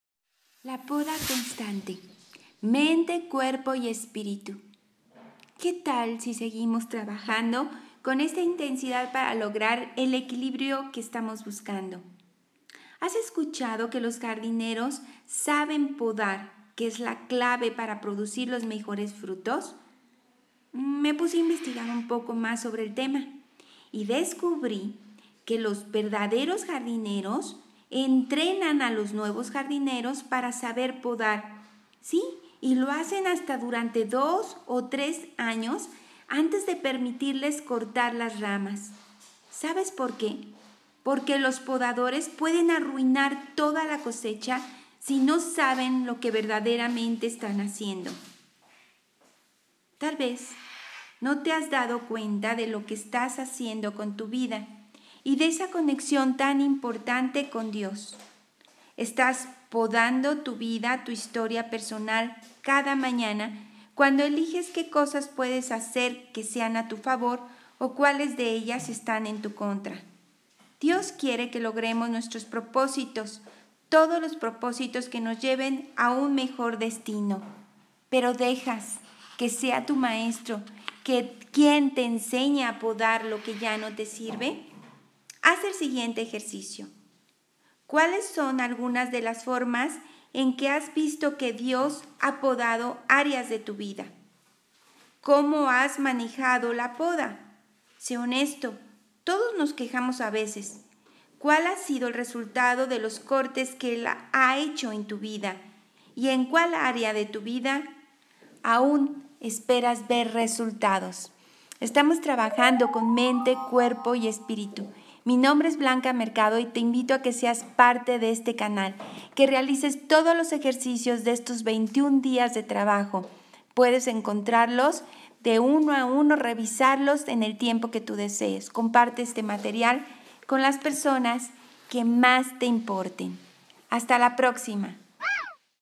¿Eres constante en tu vida?
¿Quieres saber cómo lograrlo? Realiza la siguiente actividad y sorpréndete de los resultados.